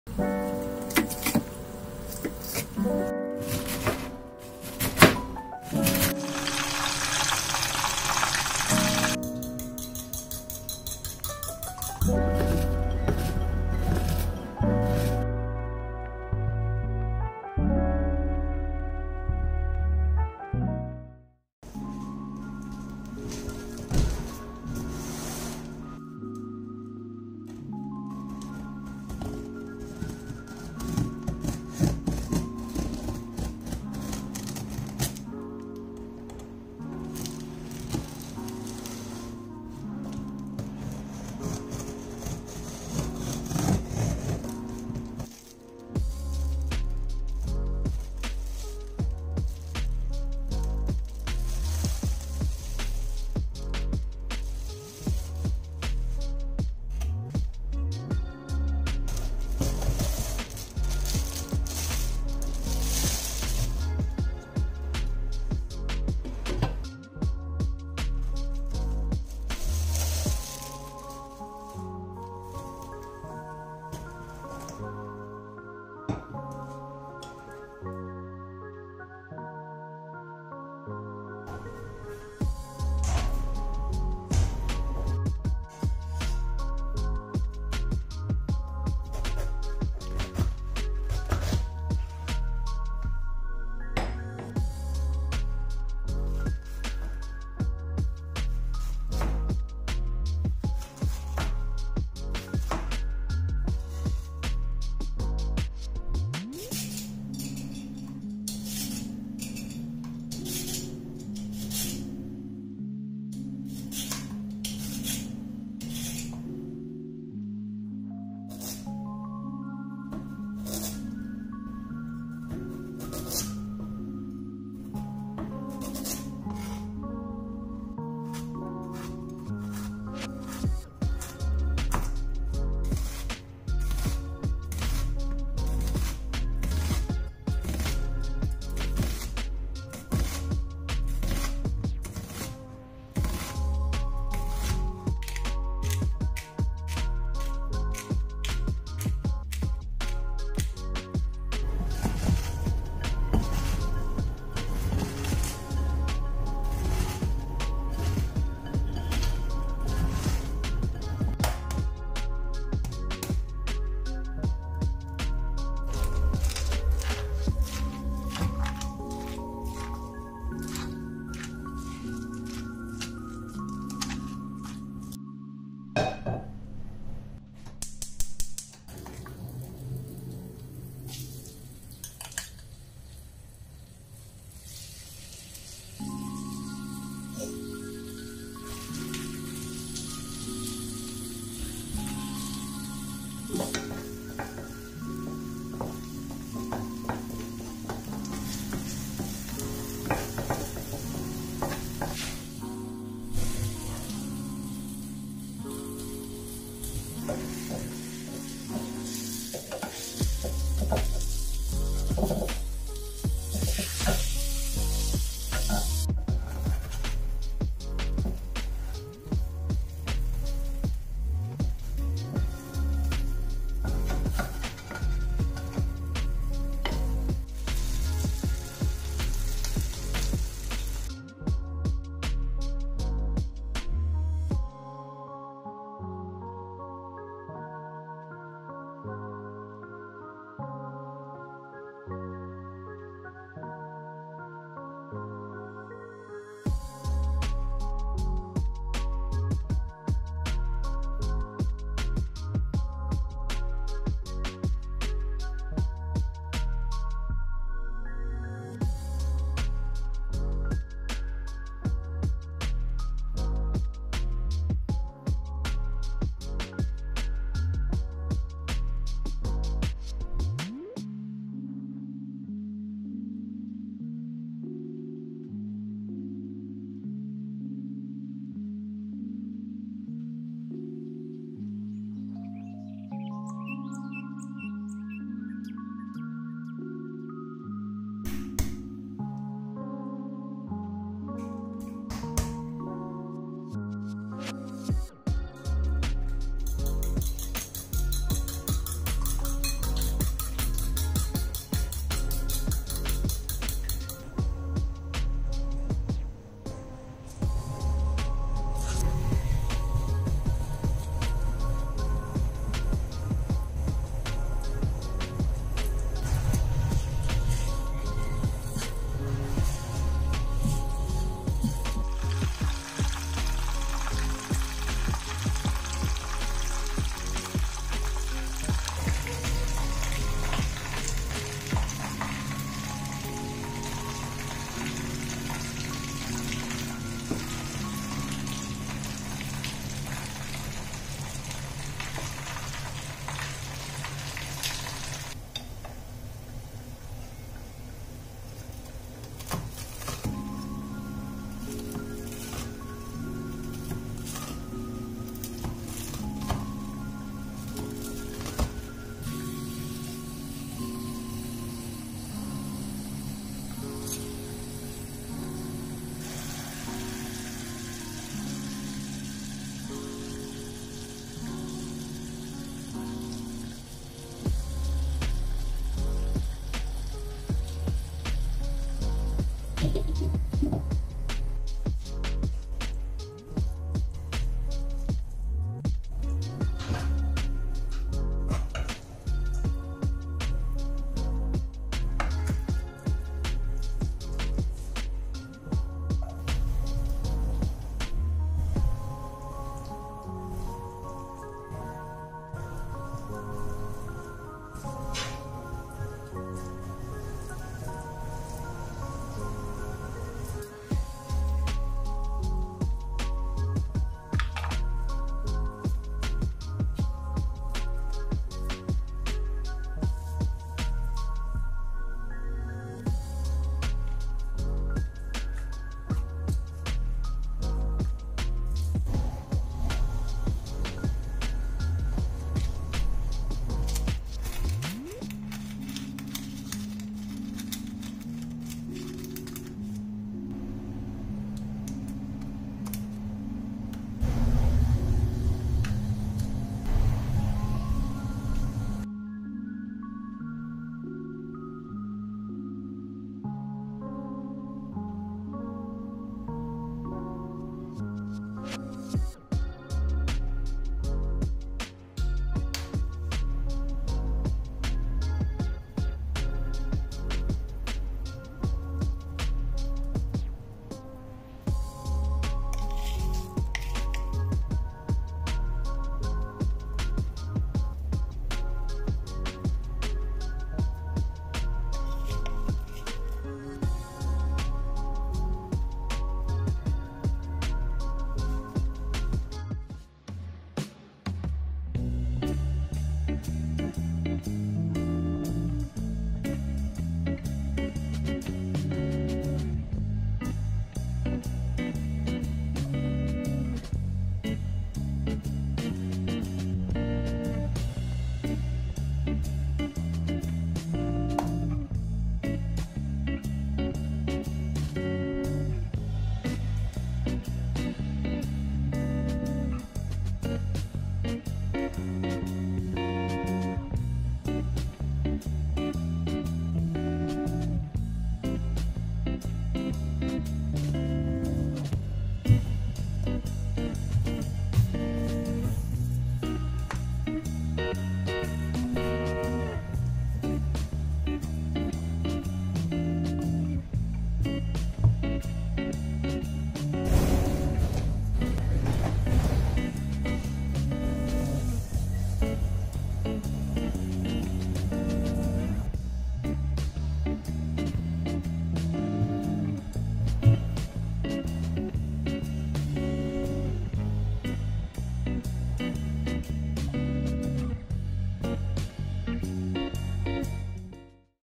Vlog #1 | Korean Street Toast breakfast | Bento Prep 🍱 | A day in my life | ASMR | cooking | SUB ❤️
Hello! I hope you are safe and healthy.

Last week I saw a video of a Korean Street Toast seller in Myeong-dong and I wanted to try making it. I'm also preparing a bento box for my boyfriend for our second anniversary. We're still on lockdown so we can't go out for a date so I'm sending him food instead. I'm also unboxing a new pot and pan that came in the mail. Adulthood made me excited about cooking stuff.

This is my first vlog and I hope you enjoy it ❤️